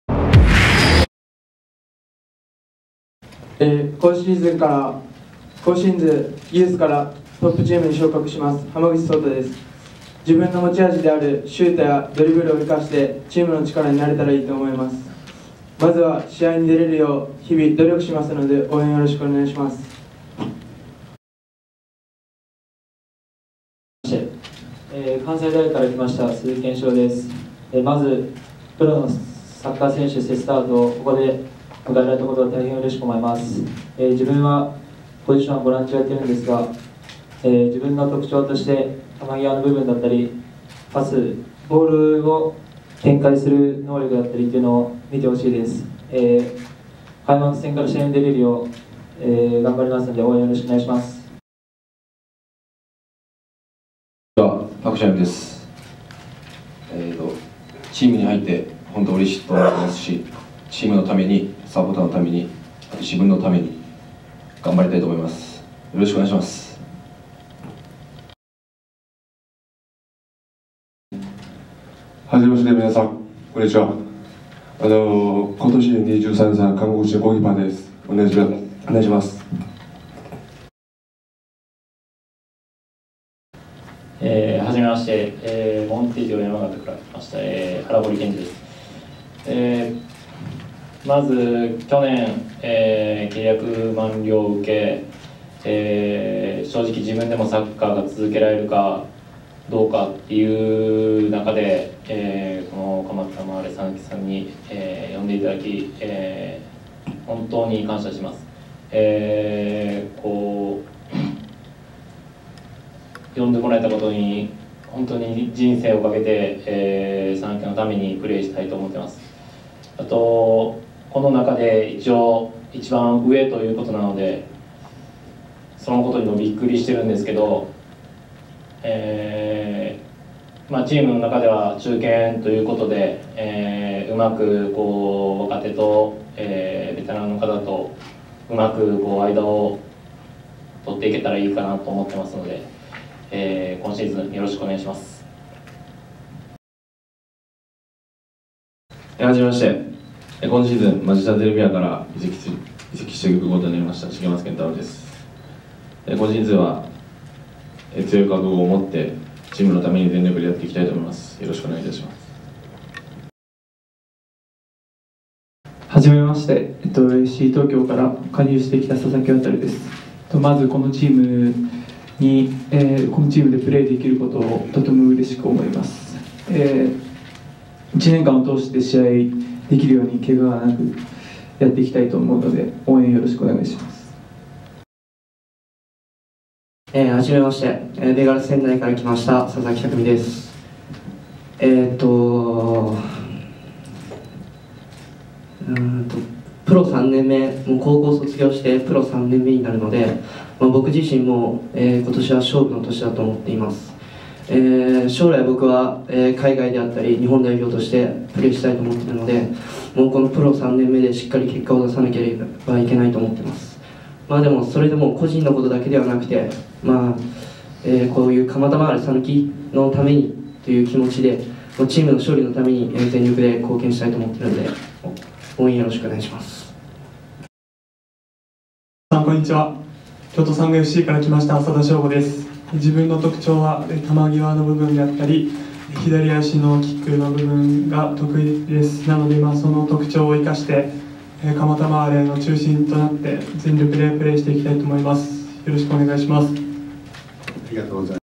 2018/01/14 2018カマタマーレ讃岐 新加入選手記者会見 選手コメント
2018年1月14日開催、2018カマタマーレ讃岐 新加入選手記者会見 選手コメント
【新加入選手 あいさつ順番】
MF 濱口 草太（ハマグチ ソウタ）
MF 鈴木 拳士郎（スズキ  ケンシロウ）
DF パク チャニョン
GK ソン ヨンミン
DF 荒堀 謙次（アラボリ  ケンジ）
FW 重松 健太郎（シゲマツ ケンタロウ）
MF 佐々木 渉（ササキ ワタル）
MF 佐々木 匠（ササキ タクミ）
DF 麻田 将吾（アサダ ショウゴ）